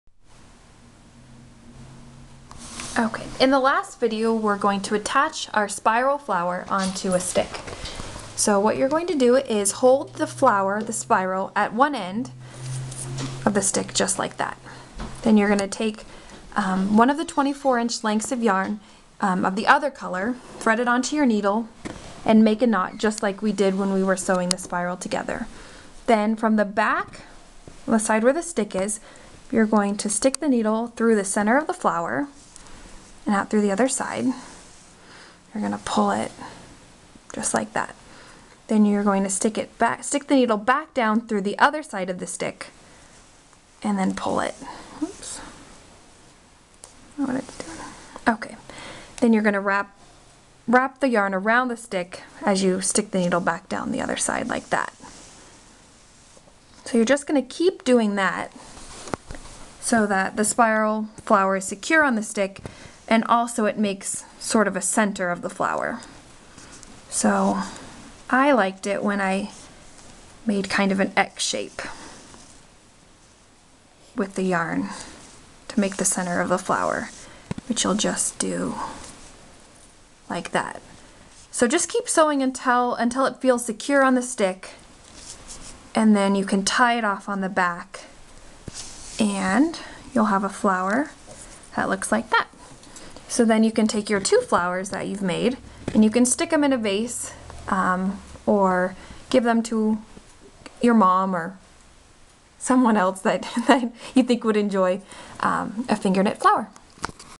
Finger Knit Flower Video #4
Video #4 for my Finger Knit Flower project. Learn how to sew your spiral of finger knitting onto a stick to make a cute flower.